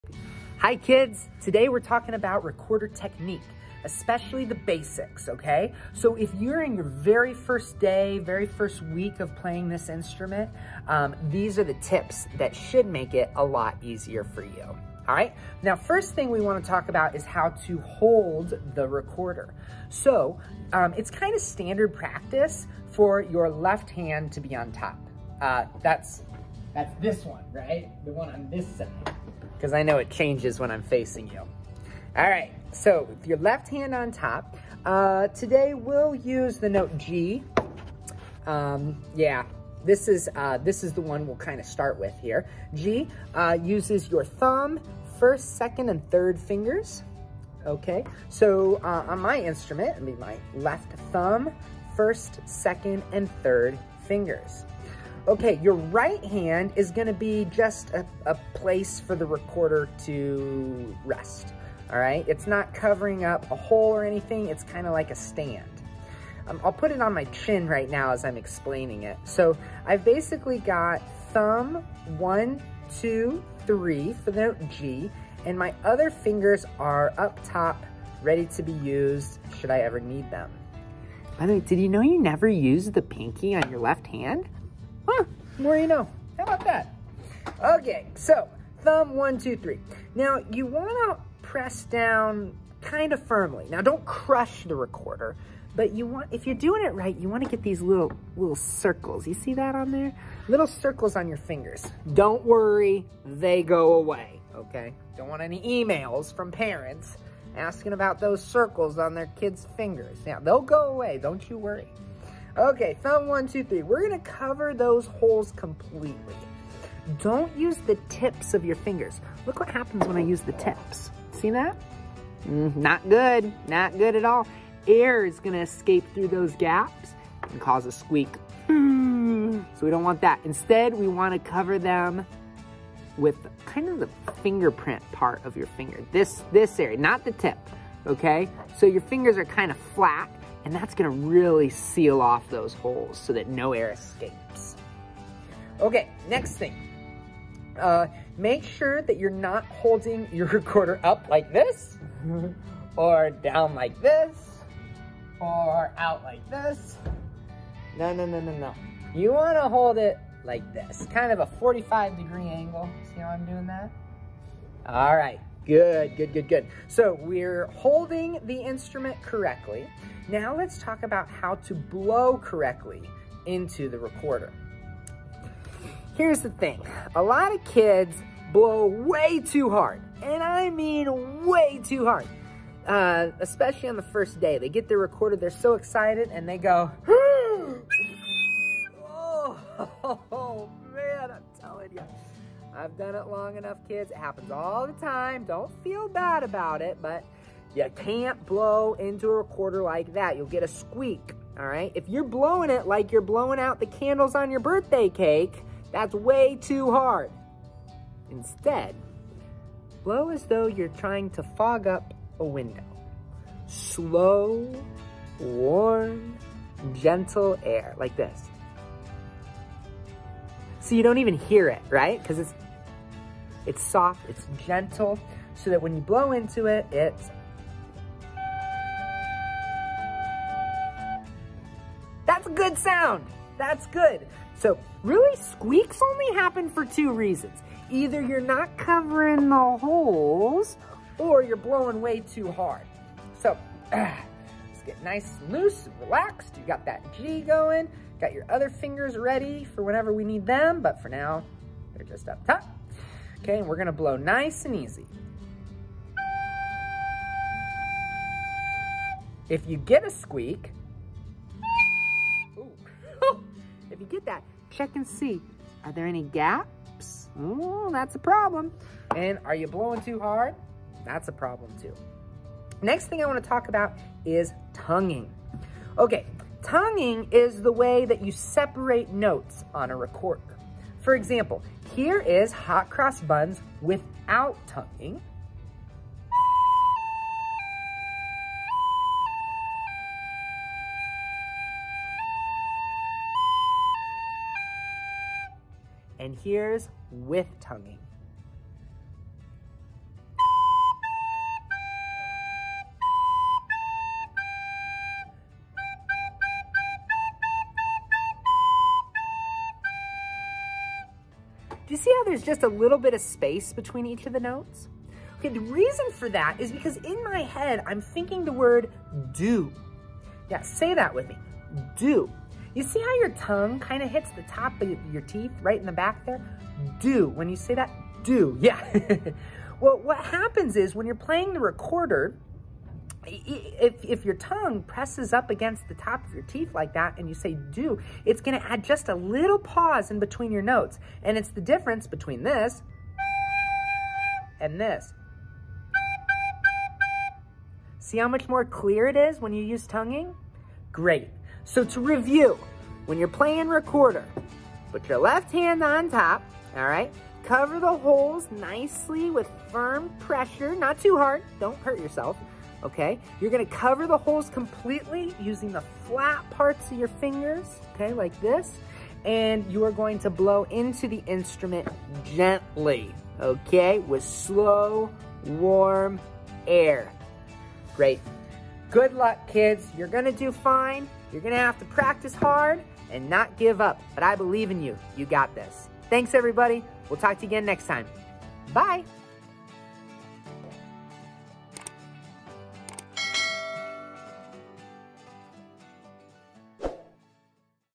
Recorder Technique- Just The Basics!
In this video Mr. Doublestein shows you how to play the recorder with good technique. This includes showing you how to hold the recorder properly, how to cover all the holes, how to blow to make good sound without squeaks, and how to use tonguing.